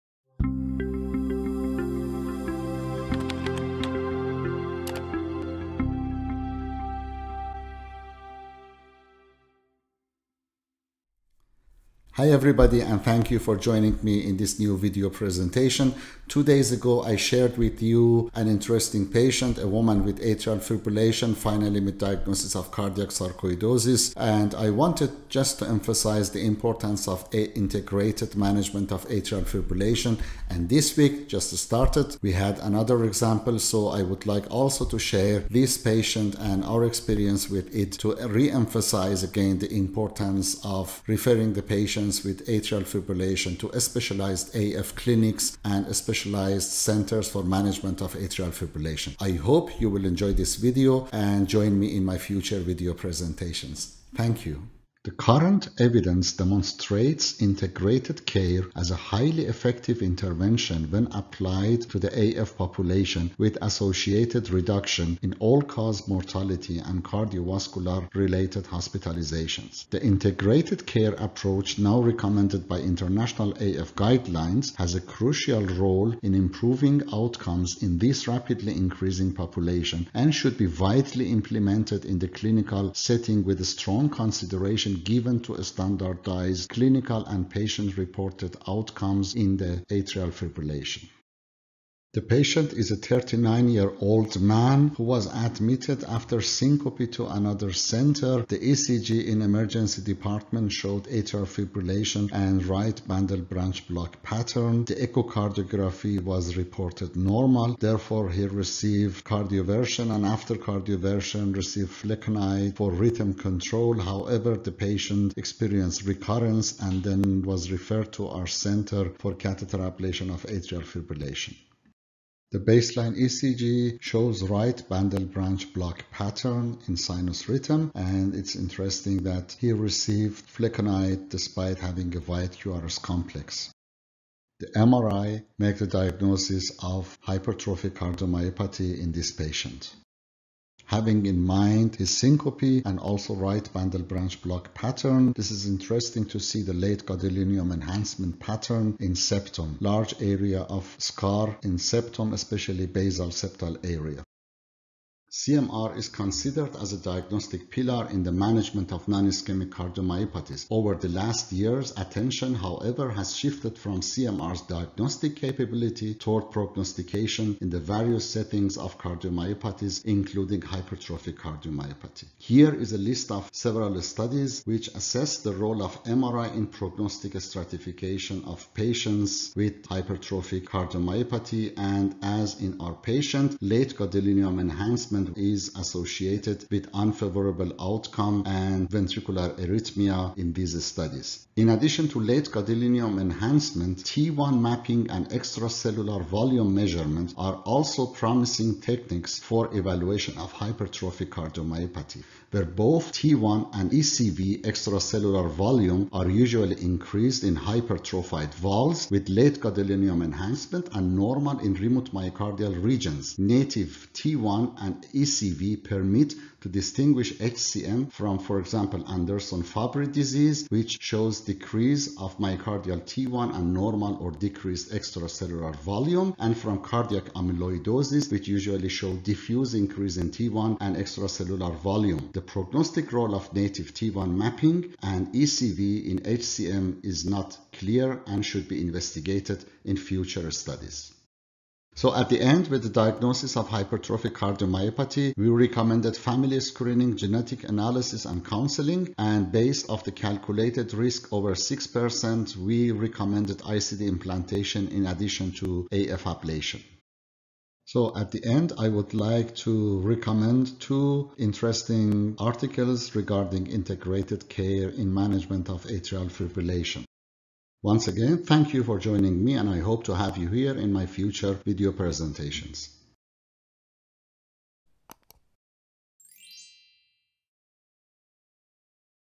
A young man with syncope and atrial fibrillation: what is the diagnosis?
A young man with atrial fibrillation and history of syncope and RBBB-pattern in ECG was referred to our center for catheter ablation of AF after failed medical therapy. The CMR revealed the diagnosis.